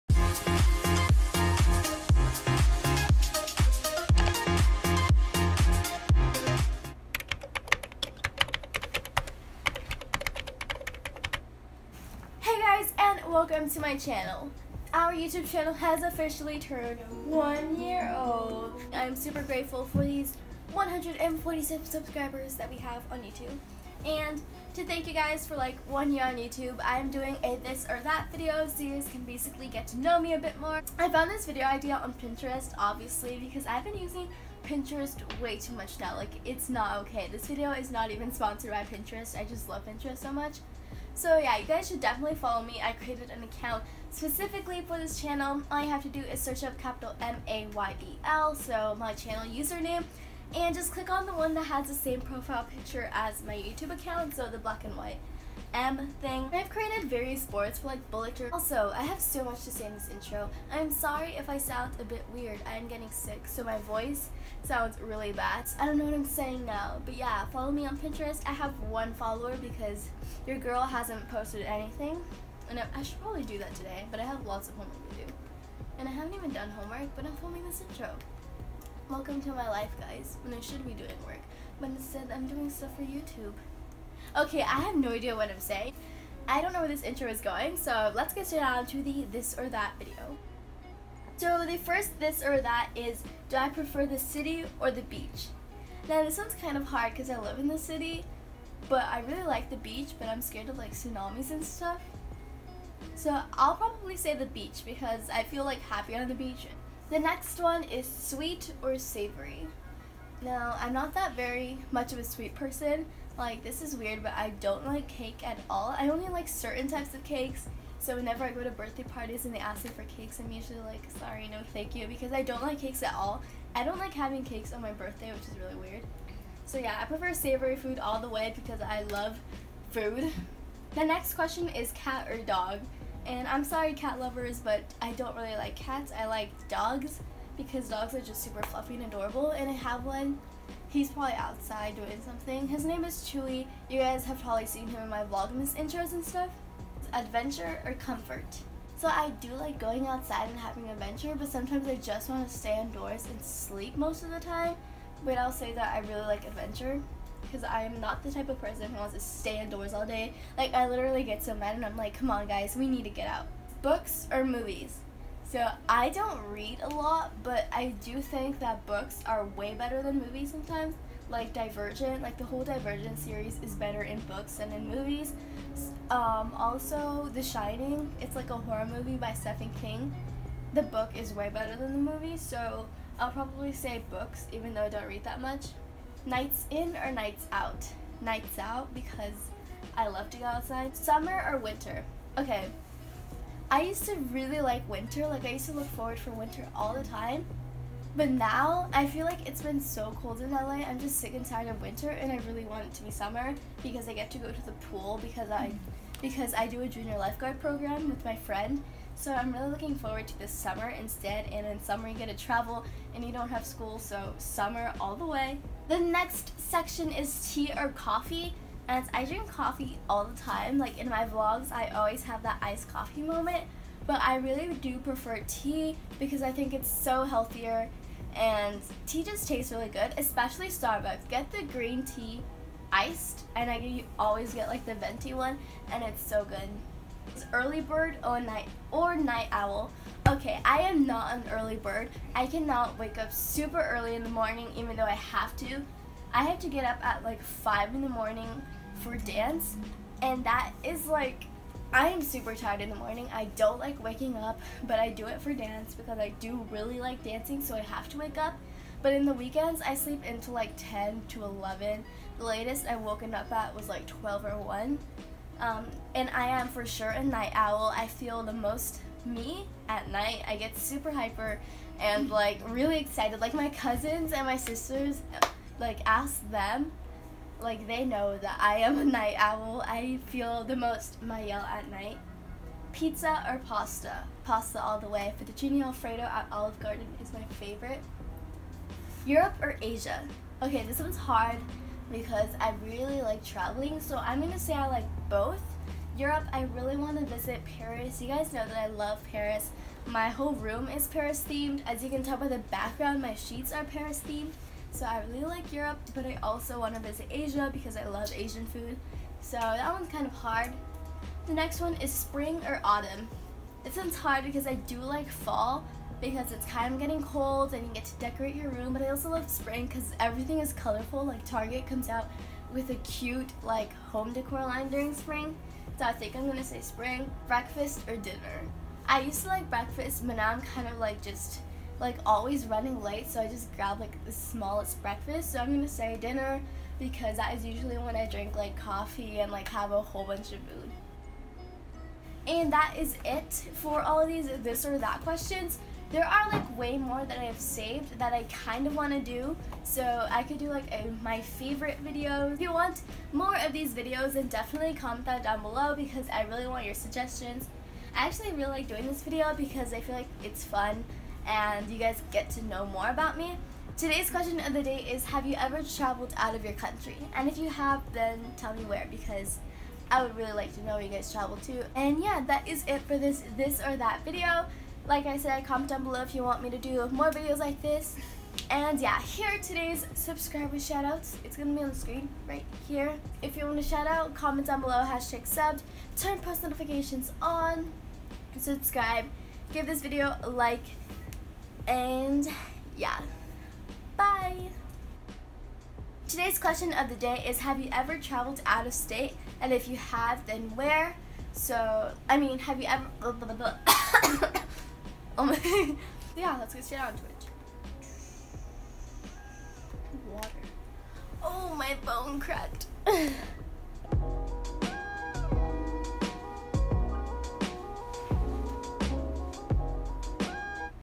Get to Know Me Tag! | Mayel
♡ Hi since my channel has officially turned 1 year old I decided to do a get to know me video! In this video, i do a this or that tag inspired by a pinterest post.

⚬ UPLOADING SCHEDULE ⚬
1) Main Channel Videos on Fridays
2) VLOGS... any other day of the week (I'm still figuring it out)

⚬ SHOUTOUT? ⚬
3) Comment